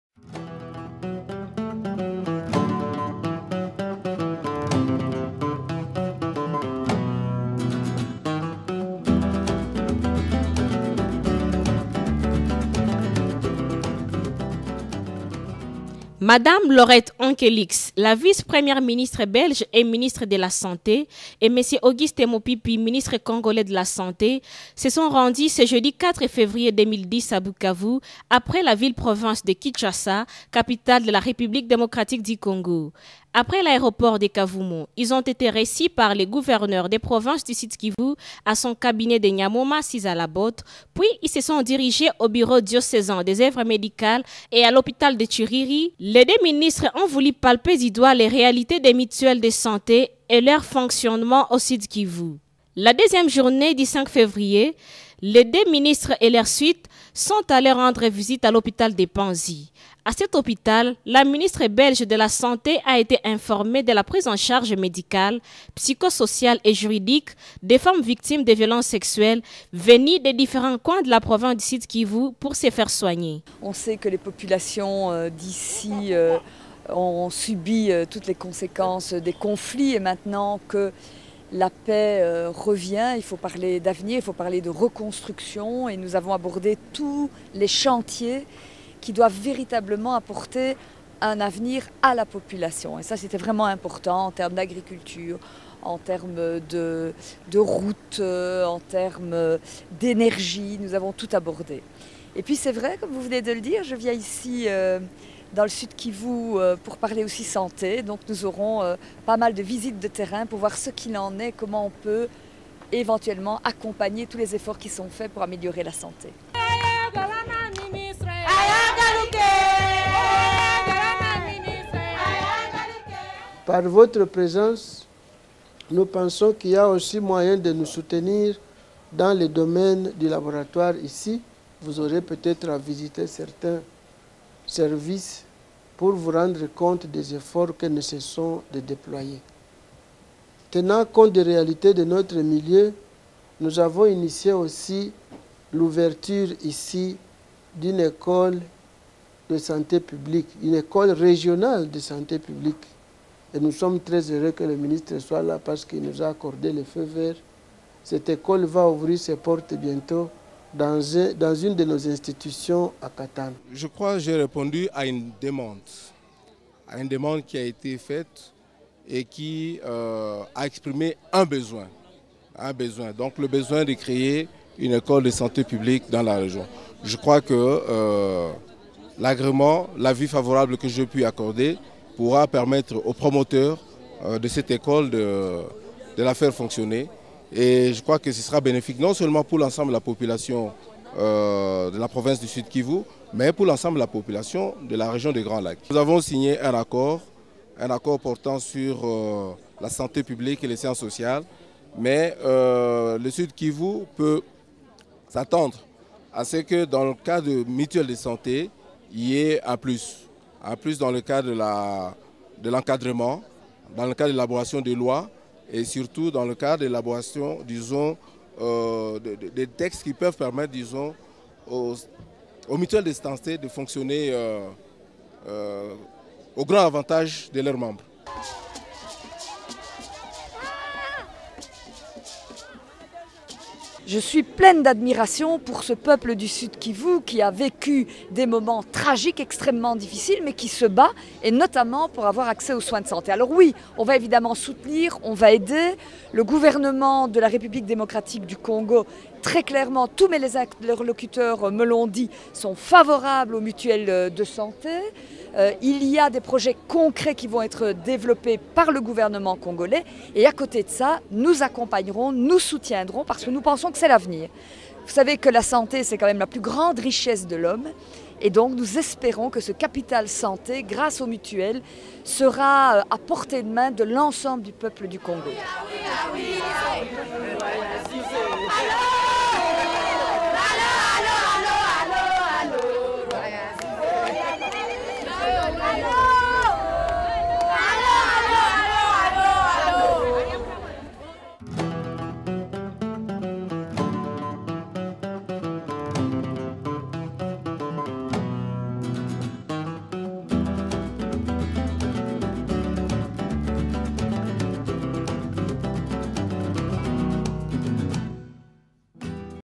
Mme L Onkelinx à Bukavu 100204
La santé, élément précieux de la préoccupation du gouvernement belge
1ère visite à Bukavu de Mme  Laurette Onkelinx, Vice-première Ministre belge et Ministre des Affaires sociales et de la Santé publique, chargée de l'Intégration sociale.
Accompagnée de Mr Auguste Mopipi, Ministre congolais de la santé, elle s'est rendue à Bukavu ce jeudi 4 février 2010 pour une mission de 2 jours, en provenance de Kinshasa.